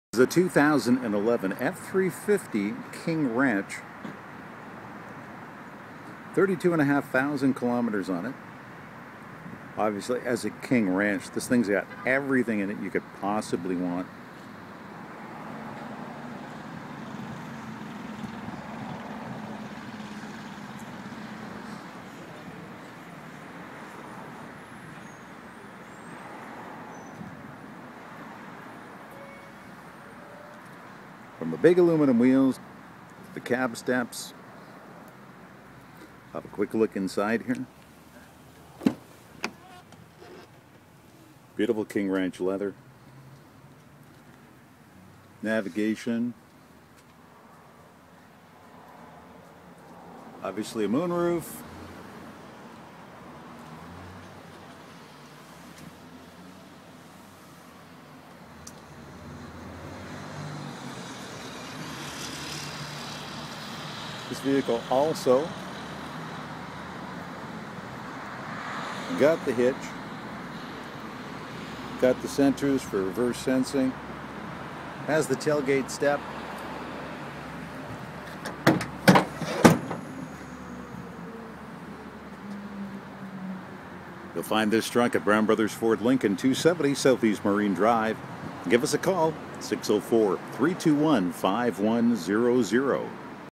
11 F350KR Rp12241.MOV
F-350 Diesel, King Ranch, Navigation, Tailgate step, upfitter switches, backup camera